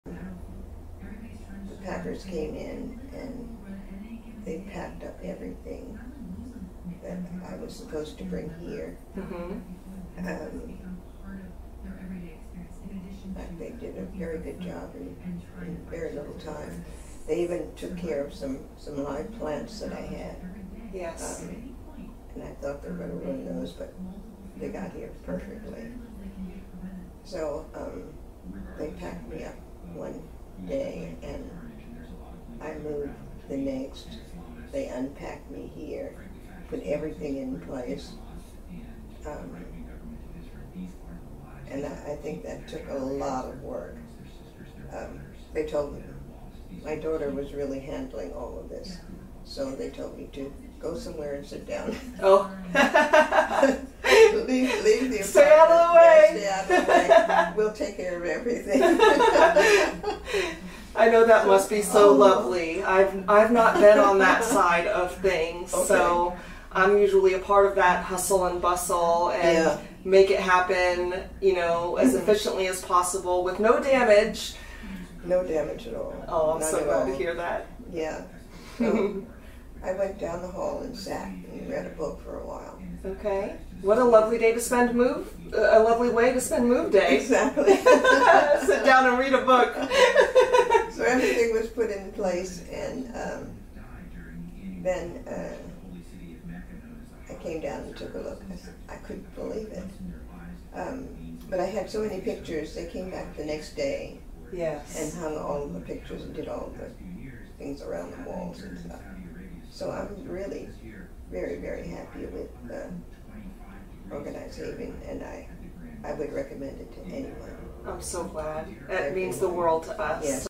Organized Haven Client Testimonial
Meet Amelita and discover her experience using Senior Move Management® services.